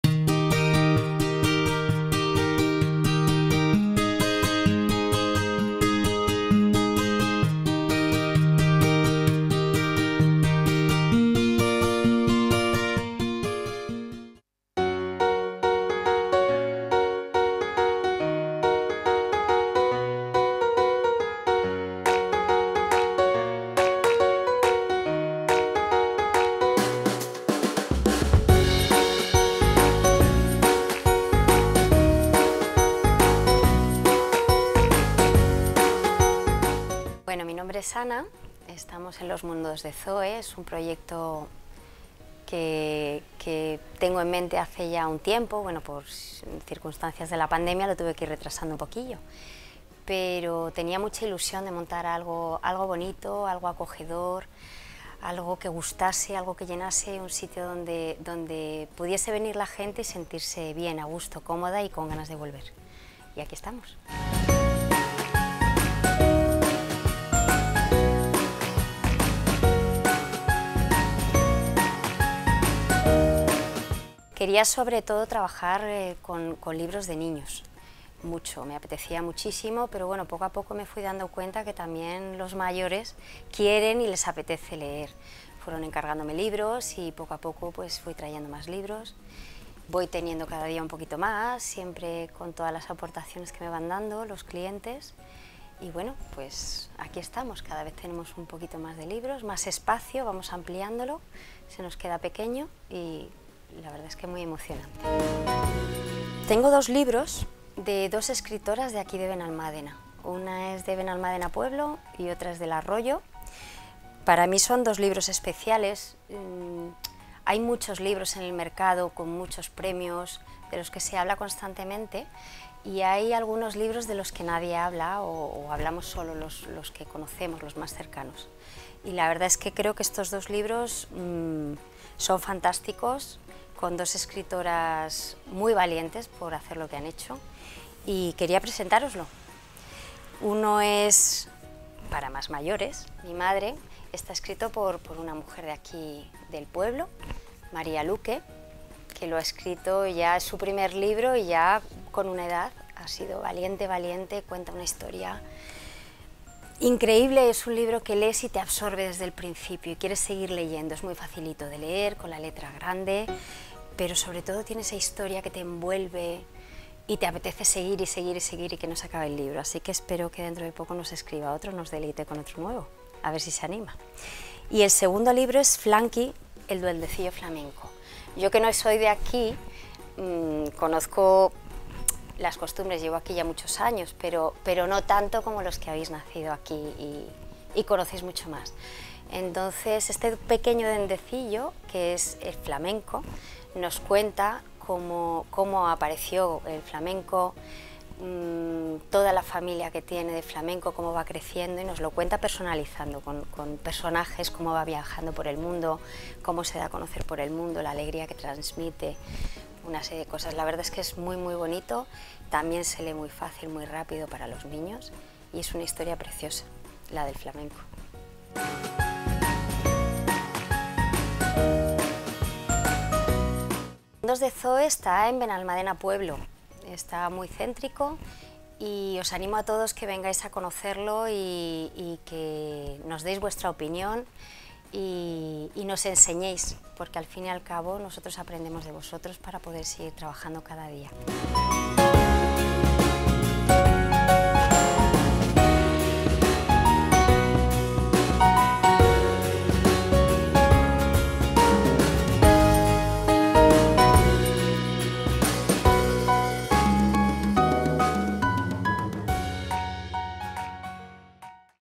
Feria del Libro Benalmádena 2021. Papelería-Librería “Los mundos de Zoe”
La situación sanitaria ha motivado que la Feria del Libro de Benalmádena de 2021 se realice íntegramente en formato digital
Ana de la Papelería-Librería “Los mundos de Zoe”, ubicada en Benalmádena Pueblo, propone acercarnos a la obra dos escritoras locales María Luque Coronado y Rocío Alba.